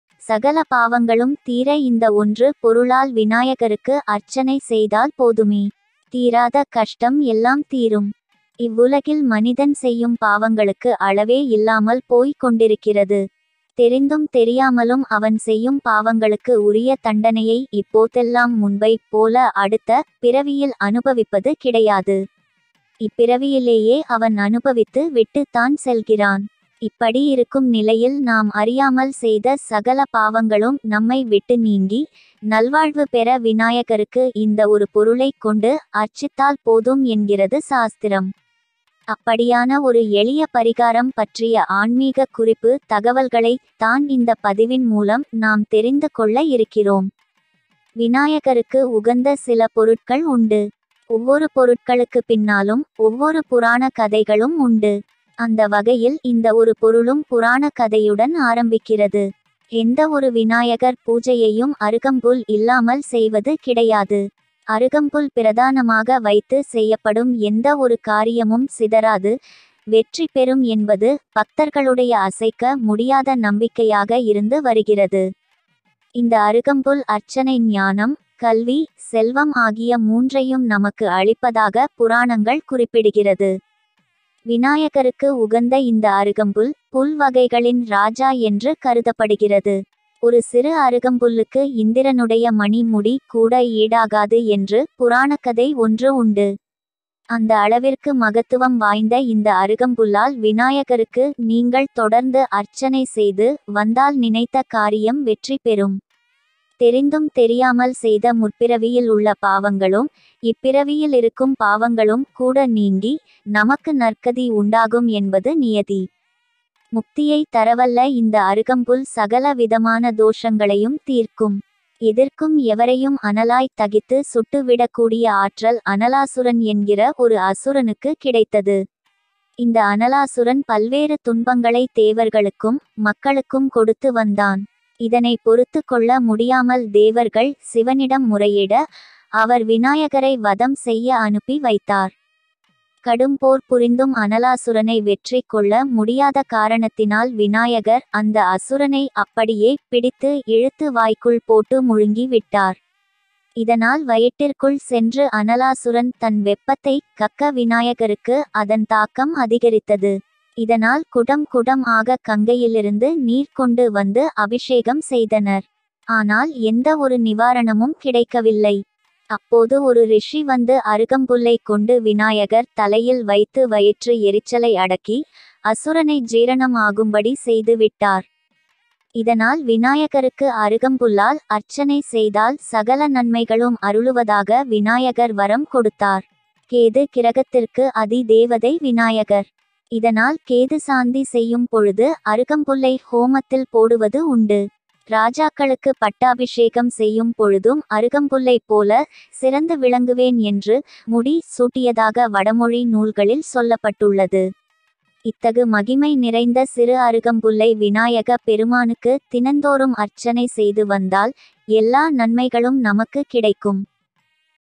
சகல பாவங்களும் தீர இந்த 1 பொருளால் விநாயகருக்கு அர்ச்சனை செய்தால் போதுமே! தீராத கஷ்டம் தீரும்
சகல பாவங்களும் தீர இந்த 1 பொருளால் விநாயகருக்கு அர்ச்சனை செய்தால் போதுமே! தீராத கஷ்டம் எல்லாம் தீரும்.

இவ்வுலகில் மனிதன் செய்யும் பாவங்களுக்கு அளவே இல்லாமல் போய்க் கொண்டிருக்கிறது. தெரிந்தும் தெரியாமலும் அவன் செய்யும் பாவங்களுக்கு உரிய தண்டனையை இப்போதெல்லாம் முன்பை போல அடுத்த பிறவியில் அனுபவிப்பது கிடையாது. இப்பிறவியிலேயே அவன் அனுபவித்து விட்டு தான் செல்கிறான். இப்படி இருக்கும் நிலையில் நாம் அறியாமல் செய்த சகல பாவங்களும் நம்மை விட்டு நீங்கி, நல்வாழ்வு பெற விநாயகருக்கு இந்த ஒரு பொருளை கொண்டு அர்ச்சித்தால் போதும் என்கிறது சாஸ்திரம். அப்படியான ஒரு எளிய பரிகாரம் பற்றிய ஆன்மீக குறிப்பு தகவல்களை தான் இந்த பதிவின் மூலம் நாம் தெரிந்து கொள்ள இருக்கிறோம்.

விநாயகருக்கு உகந்த சில பொருட்கள் உண்டு. ஒவ்வொரு பொருட்களுக்கு பின்னாலும், ஒவ்வொரு புராண கதைகளும் உண்டு. அந்த வகையில் இந்த ஒரு பொருளும் புராணக் கதையுடன் ஆரம்பிக்கிறது. எந்த ஒரு விநாயகர் பூஜையையும் அருகம்புல் இல்லாமல் செய்வது கிடையாது. அருகம்புல் பிரதானமாக வைத்து செய்யப்படும் எந்த ஒரு காரியமும் சிதறாது, வெற்றி பெறும் என்பது பக்தர்களுடைய அசைக்க முடியாத நம்பிக்கையாக இருந்து வருகிறது.

இந்த அருகம்புல் அர்ச்சனை ஞானம், கல்வி, செல்வம் ஆகிய மூன்றையும் நமக்கு அளிப்பதாக புராணங்கள் குறிப்பிடுகிறது. விநாயகருக்கு உகந்த இந்த அருகம்புல், புல் வகைகளின் ராஜா என்று கருதப்படுகிறது. ஒரு சிறு அருகம்புல்லுக்கு இந்திரனுடைய மணி முடி கூட ஈடாகாது என்று புராணக்கதை ஒன்று உண்டு. அந்த அளவிற்கு மகத்துவம் வாய்ந்த இந்த அருகம்புல்லால் விநாயகருக்கு நீங்கள் தொடர்ந்து அர்ச்சனை செய்து வந்தால் நினைத்த காரியம் வெற்றி பெறும். தெரிந்தும் தெரியாமல் செய்த முற்பிறவியில் உள்ள பாவங்களும், இப்பிறவியில் இருக்கும் பாவங்களும் கூட நீங்கி, நமக்கு நற்கதி உண்டாகும் என்பது நியதி.

முக்தியை தரவல்ல இந்த அருகம்புல் சகல விதமான தோஷங்களையும் தீர்க்கும். எதிர்க்கும் எவரையும் அனலாய் தகித்து சுட்டு விடக் கூடிய ஆற்றல் அனலாசுரன் என்கிற ஒரு அசுரனுக்கு கிடைத்தது. இந்த அனலாசுரன் பல்வேறு துன்பங்களை தேவர்களுக்கும், மக்களுக்கும் கொடுத்து வந்தான். இதனை பொறுத்துக் கொள்ள முடியாமல் தேவர்கள் சிவனிடம் முறையிட, அவர் விநாயகரை வதம் செய்ய அனுப்பி வைத்தார்.

கடும் போர் புரிந்தும் அனலாசுரனை வெற்றி கொள்ள முடியாத காரணத்தினால் விநாயகர் அந்த அசுரனை அப்படியே பிடித்து இழுத்து வாய்க்குள் போட்டு முழுங்கி விட்டார். இதனால் வயிற்றிற்குள் சென்று அனலாசுரன் தன் வெப்பத்தை கக்க விநாயகருக்கு அதன் தாக்கம் அதிகரித்தது. இதனால் குடம் குடம் ஆக கங்கையிலிருந்து நீர் கொண்டு வந்து அபிஷேகம் செய்தனர். ஆனால் எந்த ஒரு நிவாரணமும் கிடைக்கவில்லை

அப்போது ஒரு ரிஷி வந்து அருகம்புல்லை கொண்டு விநாயகர் தலையில் வைத்து வயிற்று எரிச்சலை அடக்கி, அசுரனை ஜீரணம் ஆகும்படி செய்து விட்டார். இதனால் விநாயகருக்கு அருகம்புல்லால் அர்ச்சனை செய்தால் சகல நன்மைகளும் அருளுவதாக விநாயகர் வரம் கொடுத்தார். கேது கிரகத்திற்கு அதி தேவதை விநாயகர். இதனால் கேது சாந்தி செய்யும் பொழுது அருகம்புல்லை ஹோமத்தில் போடுவது உண்டு. ராஜாக்களுக்கு பட்டாபிஷேகம் செய்யும் பொழுதும் அருகம்புல்லை போல சிறந்து விளங்குவேன் என்று முடி சூட்டியதாக வடமொழி நூல்களில் சொல்லப்பட்டுள்ளது. இத்தகு மகிமை நிறைந்த சிறு அருகம்புல்லை விநாயகப் பெருமானுக்கு தினந்தோறும் அர்ச்சனை செய்து வந்தால், எல்லா நன்மைகளும் நமக்கு கிடைக்கும். @selviyinselvan